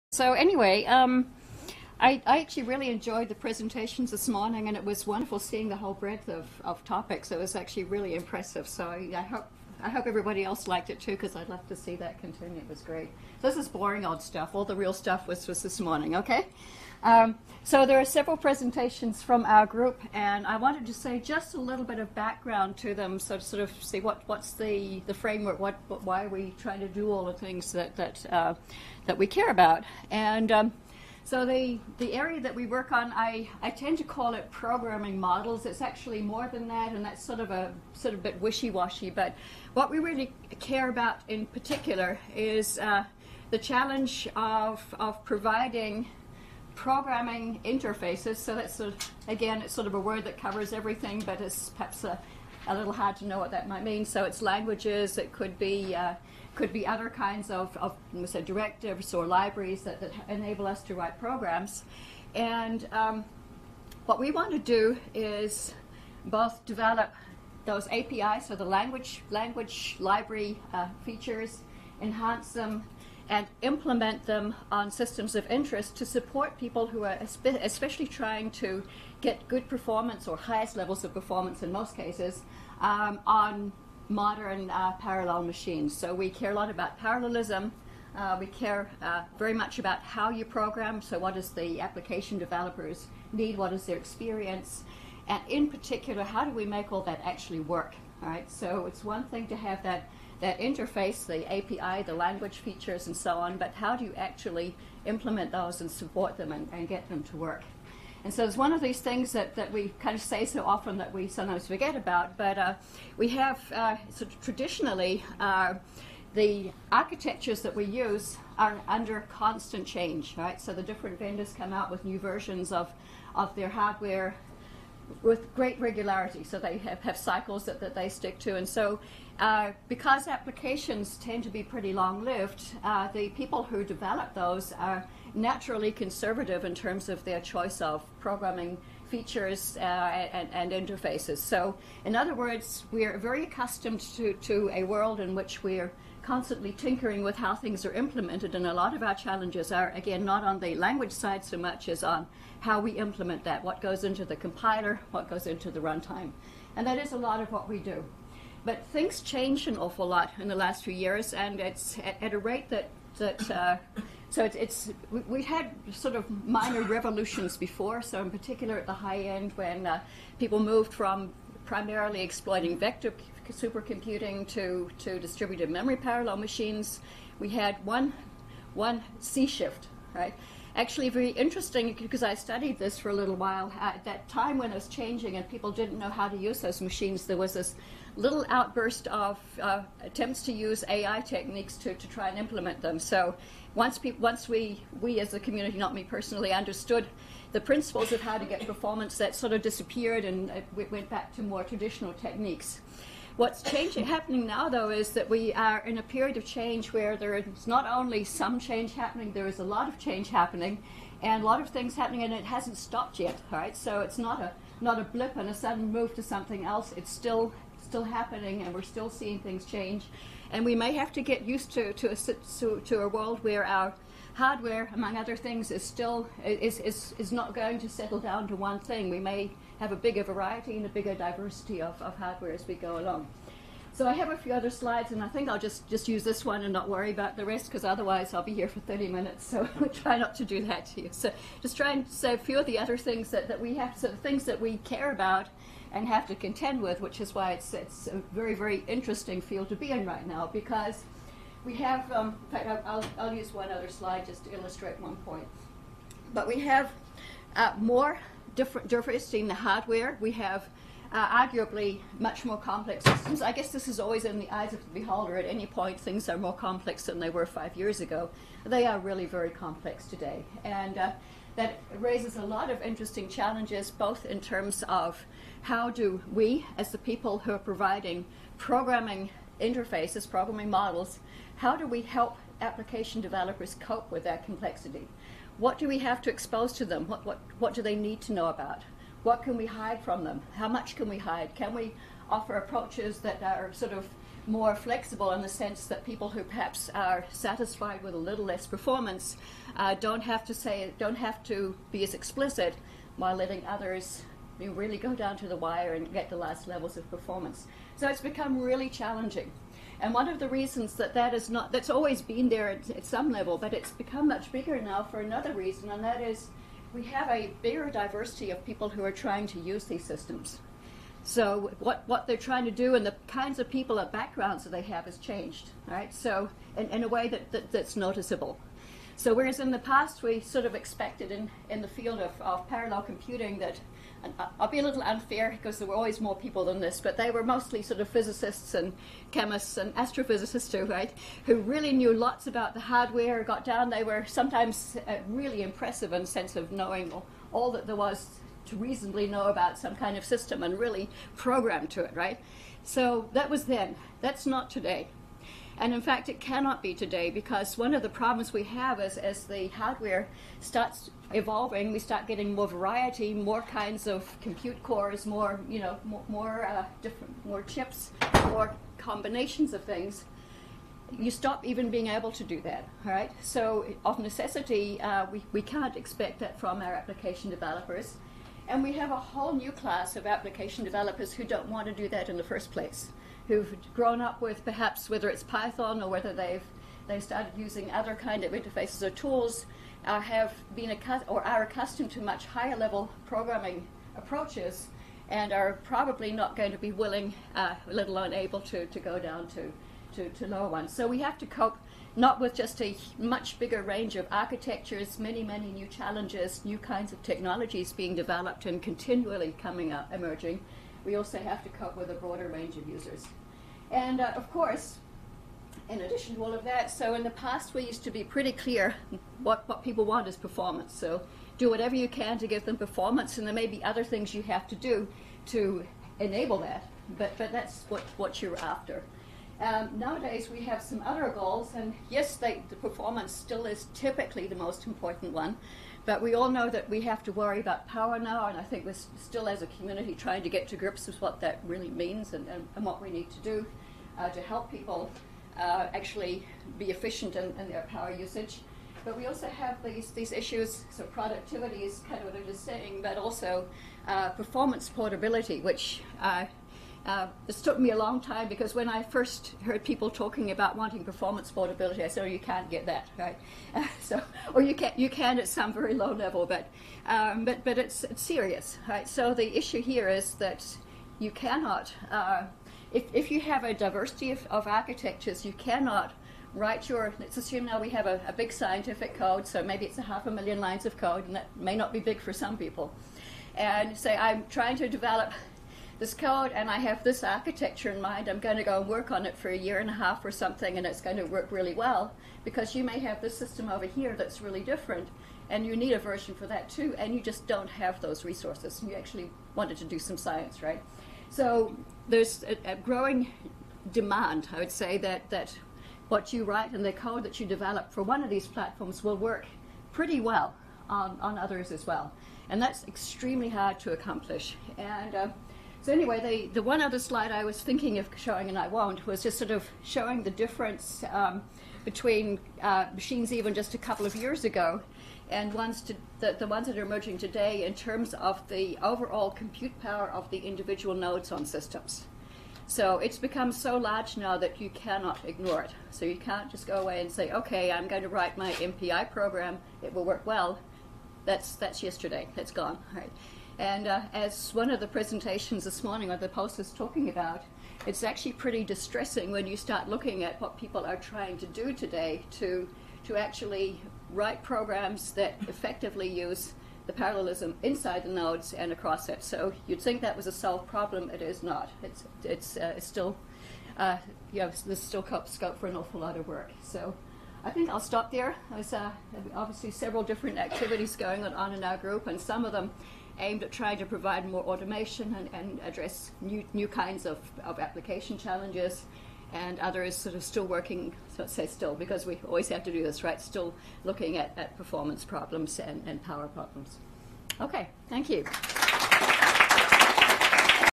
IACS Research Day Faculty Talks: Barbara Chapman
Architectures and Workloads are Changing: Can Our Programming Models Evolve?, Barbara Chapman